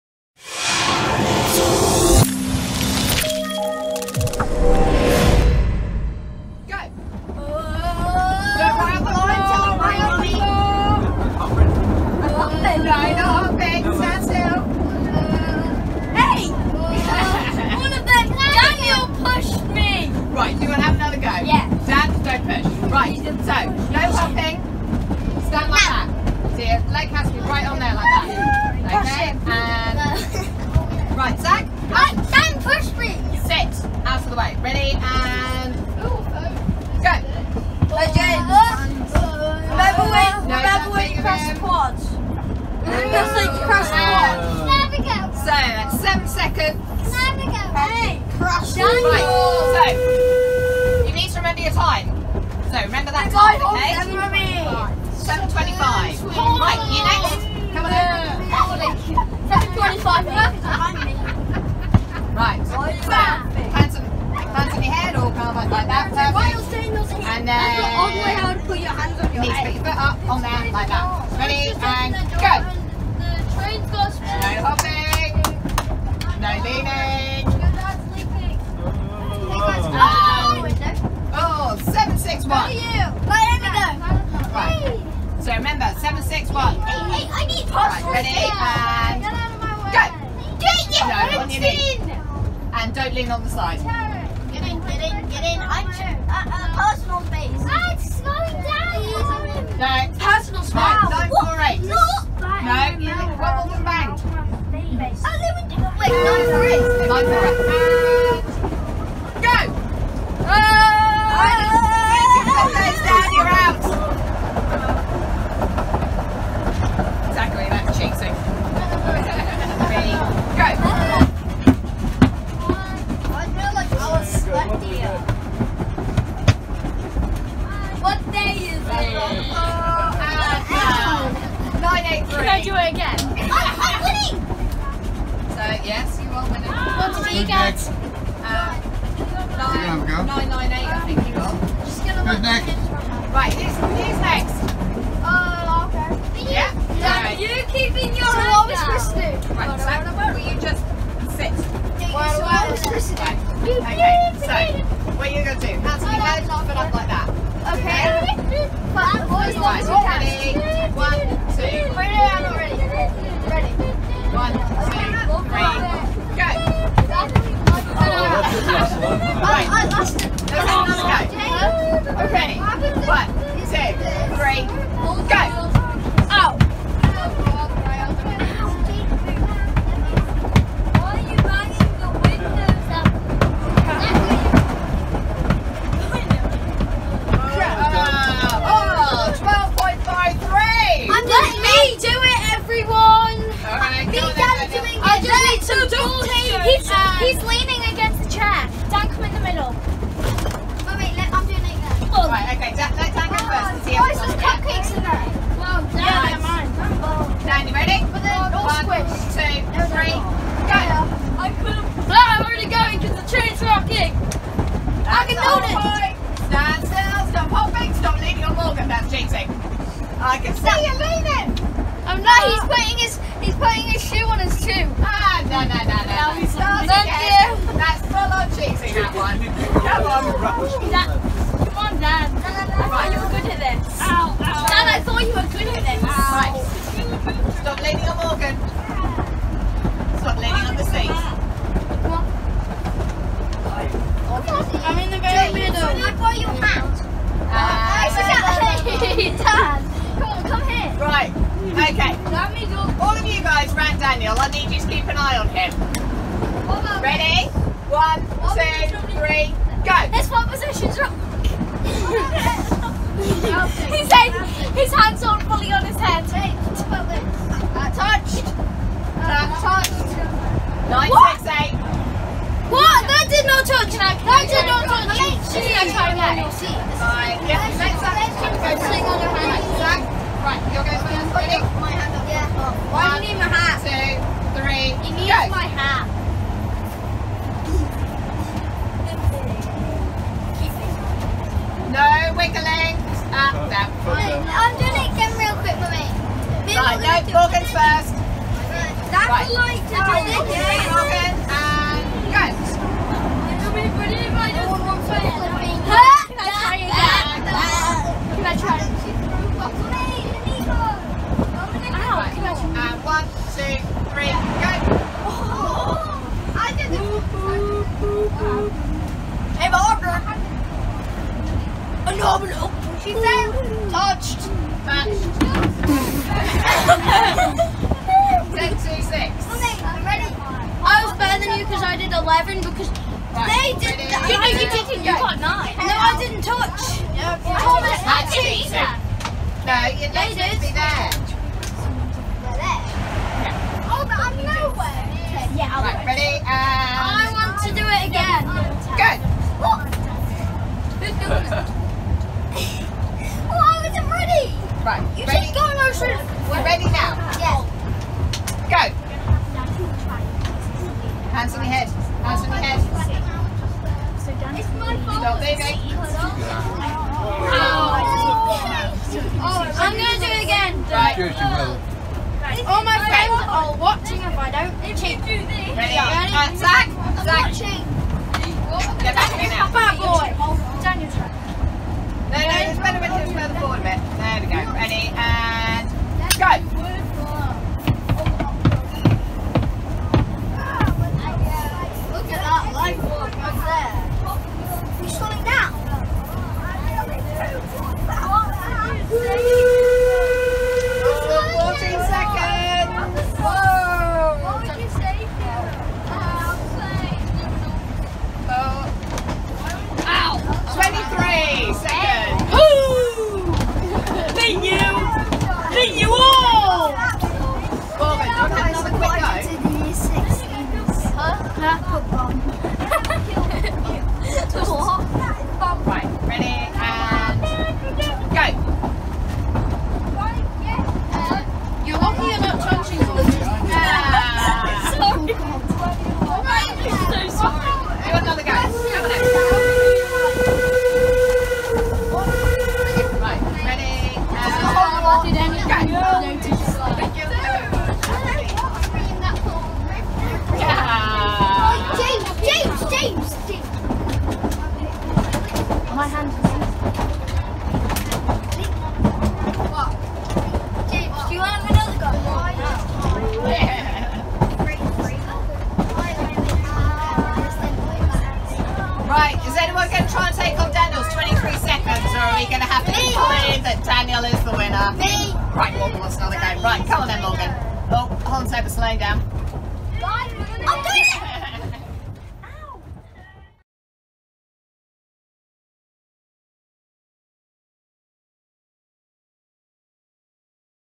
1st Marown & 1st Douglas Train Trip
A trip to the beach by steam train - August 2020